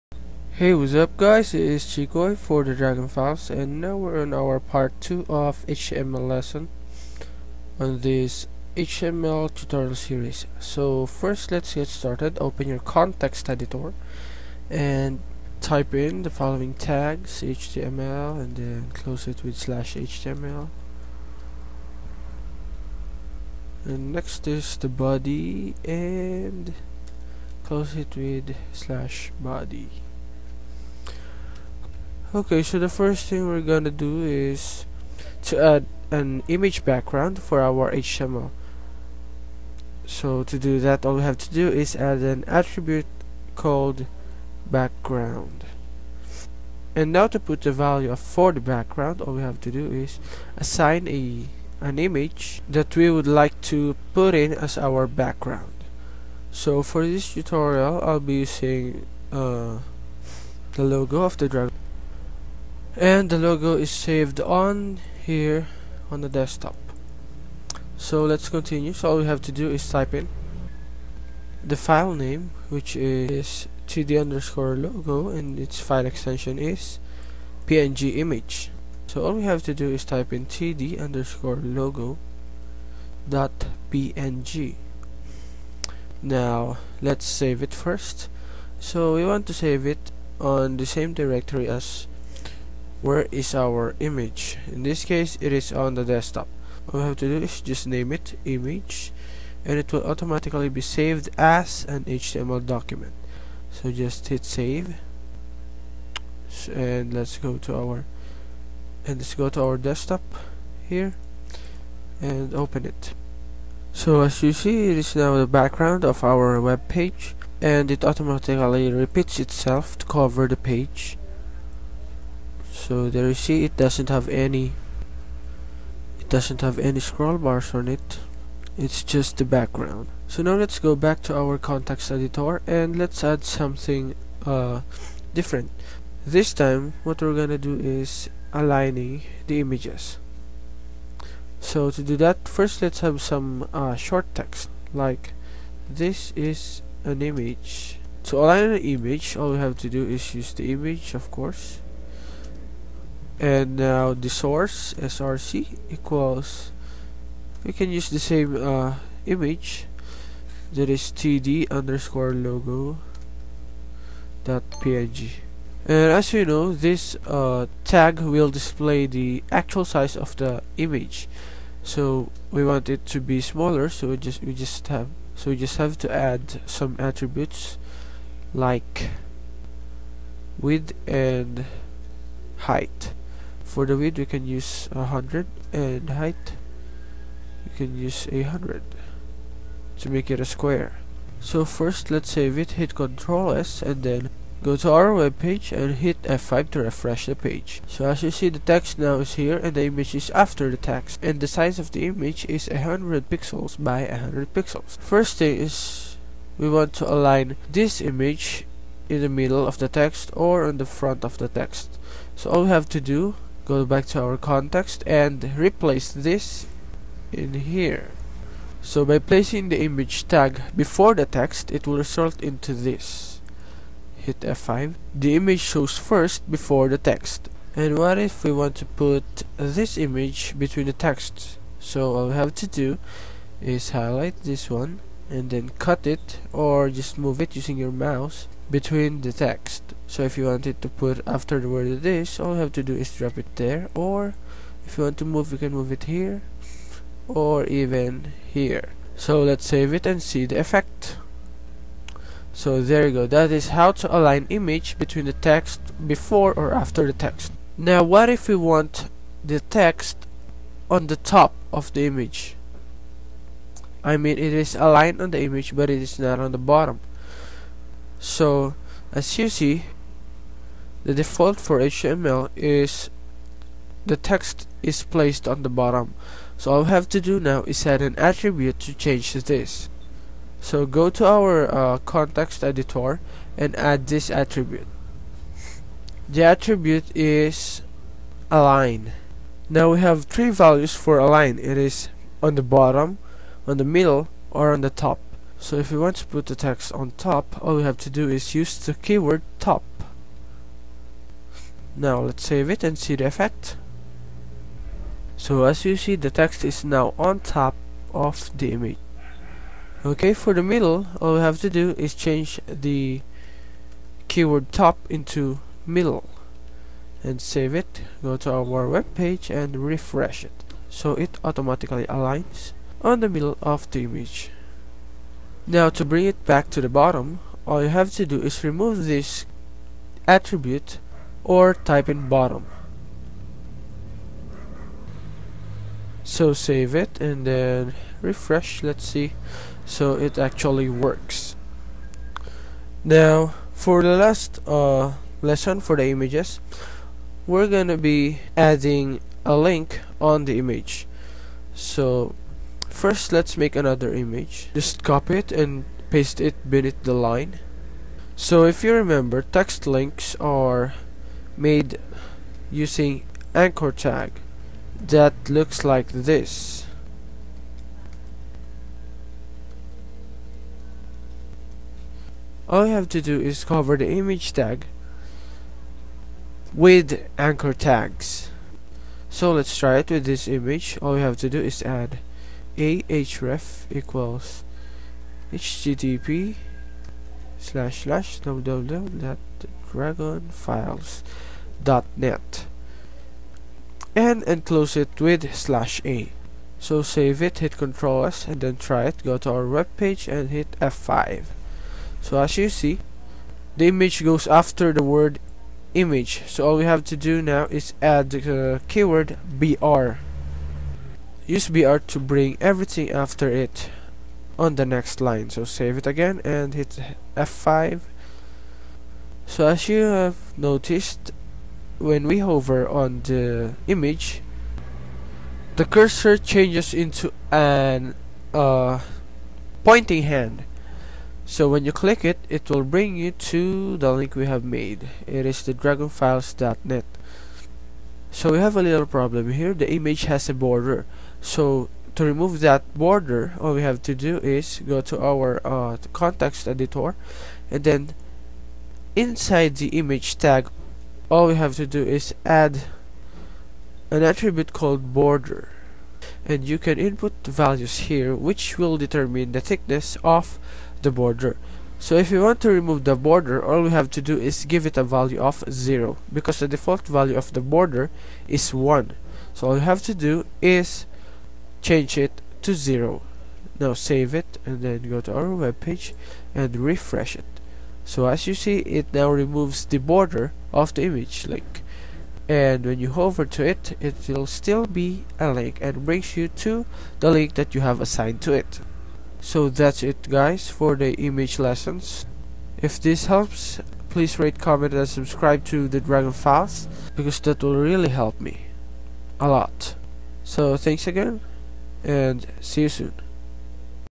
HTML Tutorial 10 Adding images part 2
learn how to align images, putting up a background image, and making an image link.